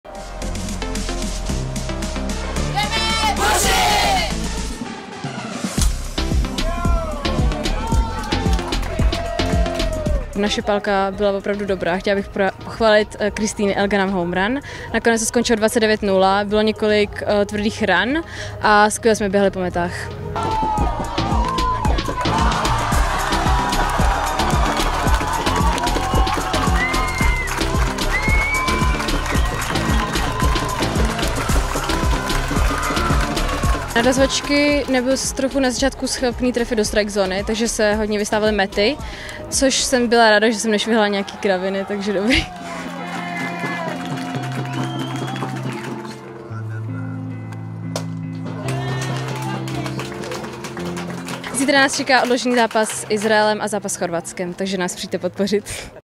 ME juniorek 2023: Ohlasy s Karolínou Beránkovou
Bezchybná obrana a skvělý útok. Prvometařka Karolína Beránková hodnotí druhou výhru na šampionátu, kdy český tým porazil Slovensko 29:0.